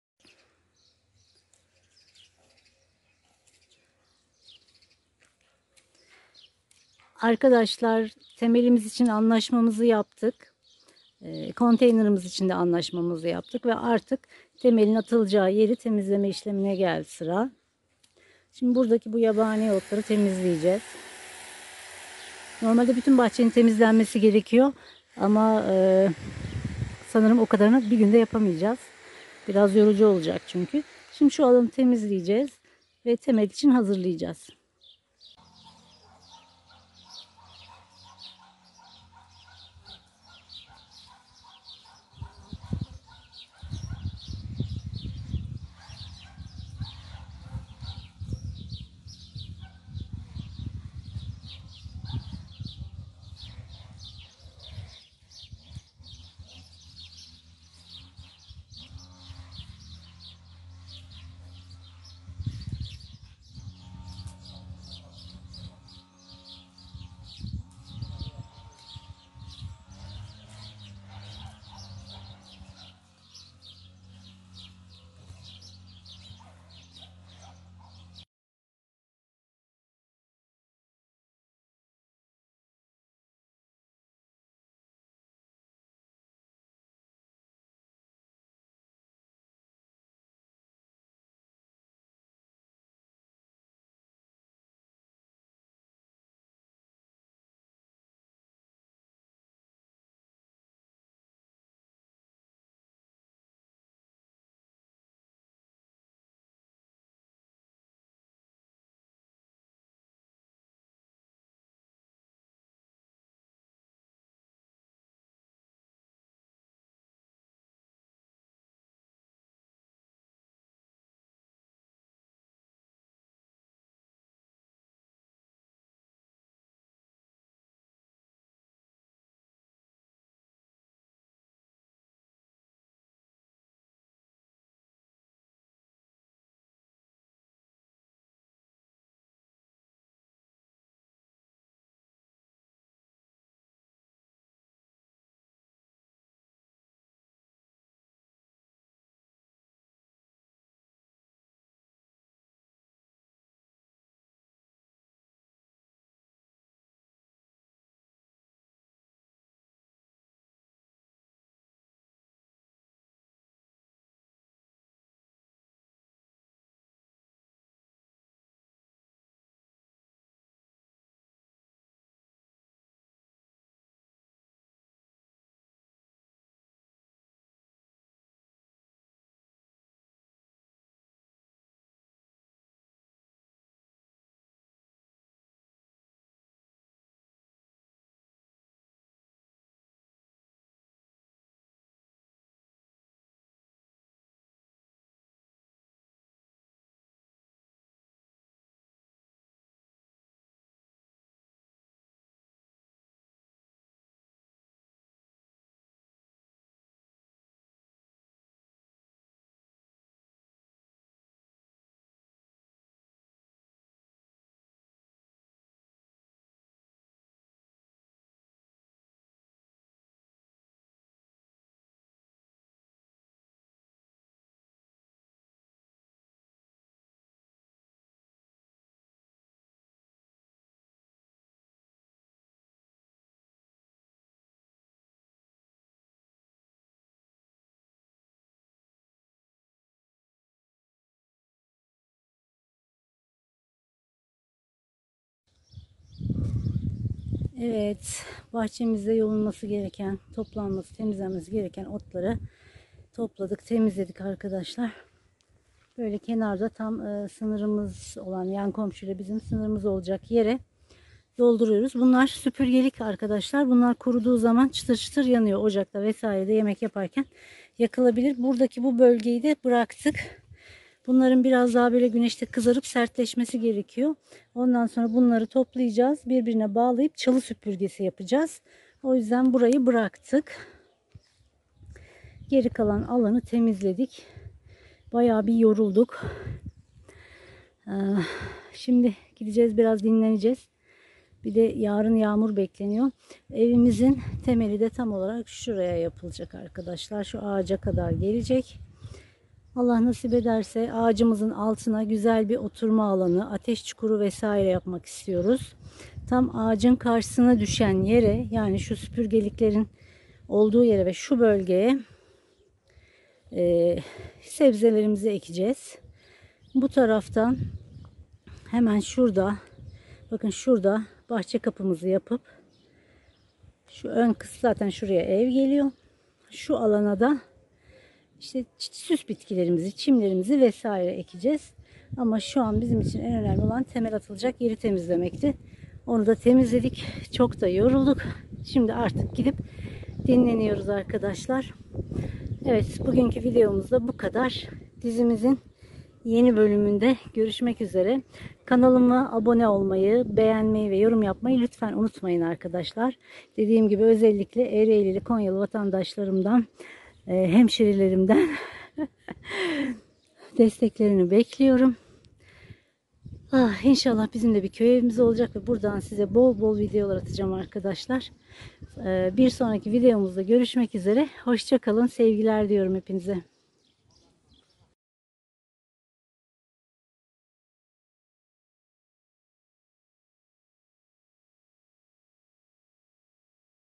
İstanbul'dan Köye Dönüş 6.Bölüm / Konya Ereğli'ye Ev Yaptırıyoruz 🏠Temel İçin Arsamızı Temizledik
Merhabalar arkadaşlar,
Evimizin temeli için bahçemizde çalışmalara  başladık.